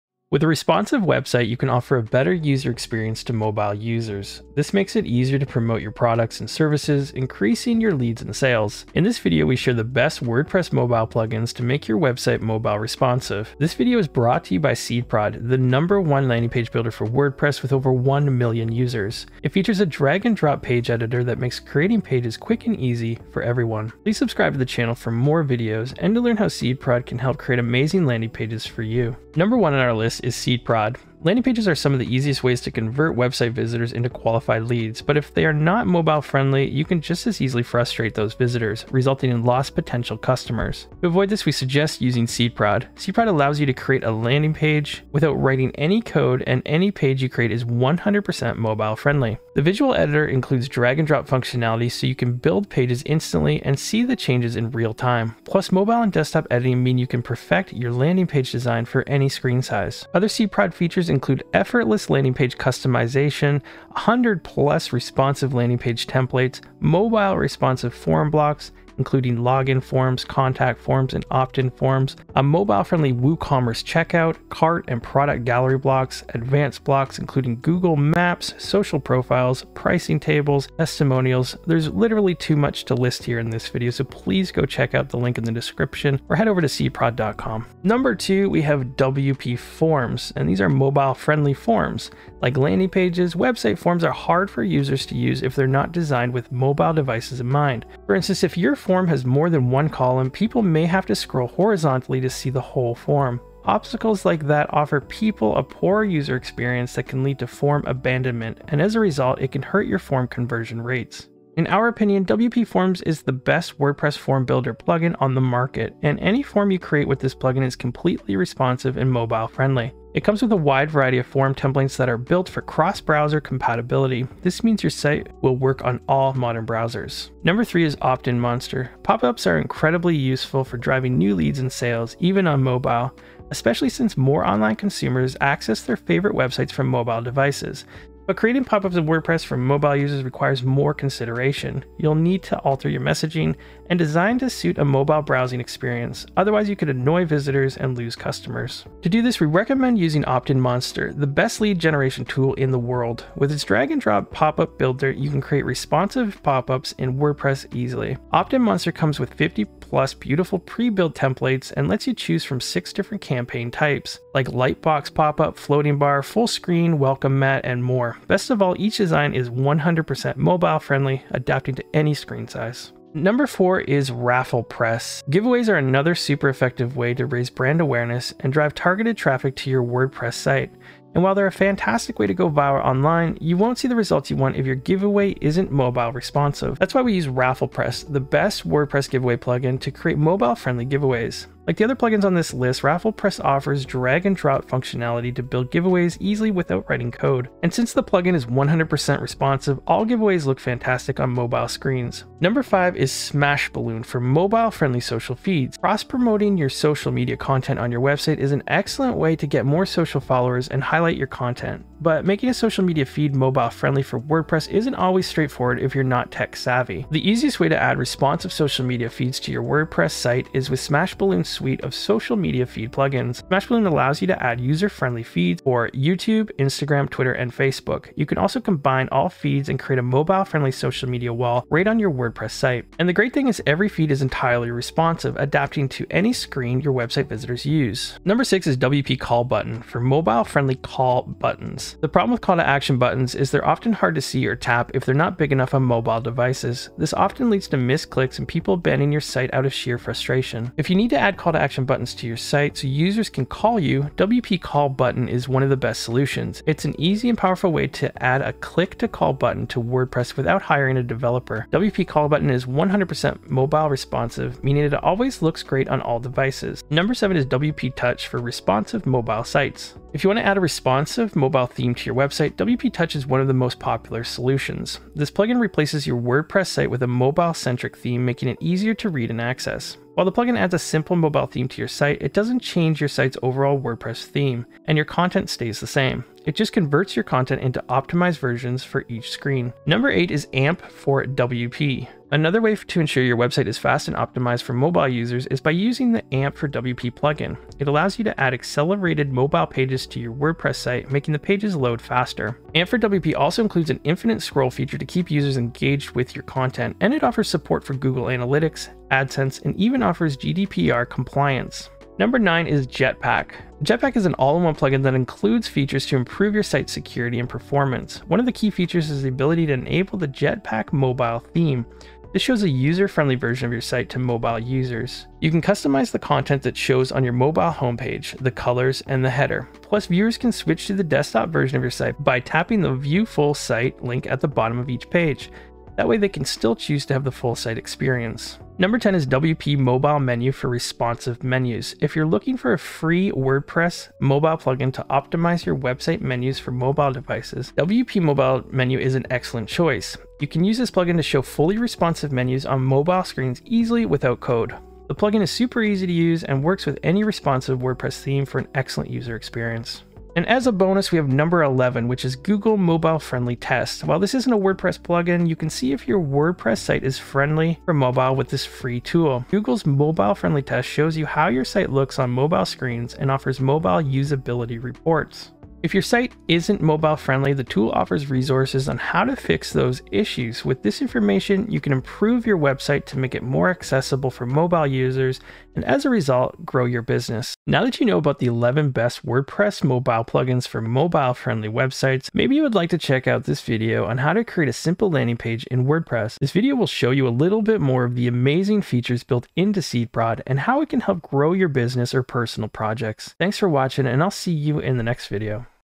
11 Best WordPress Mobile Plugins for Mobile Friendly Websites
With a responsive website, you can offer a better user experience to mobile users. This makes it easier to promote your products and services, increasing your leads and sales.

In this video, we share the best WordPress mobile plugins to make your website mobile responsive.

Timestamps:
00:00 - Introduction
00:29 - SeedProd
01:27 - WPForms
02:07 - OptinMonster
02:52 - RafflePress
03:22 - Smash Balloon
03:58 - WP Call Button
04:26 - WPTouch
04:49 - AMP for WP
05:12 - Jetpack
05:38 - WP Mobile Menu
06:00 - Google Mobile-Friendly Test
06:27 - Outro

Check out the article for the 11 Best WordPress Mobile Plugins for Mobile Friendly Websites here: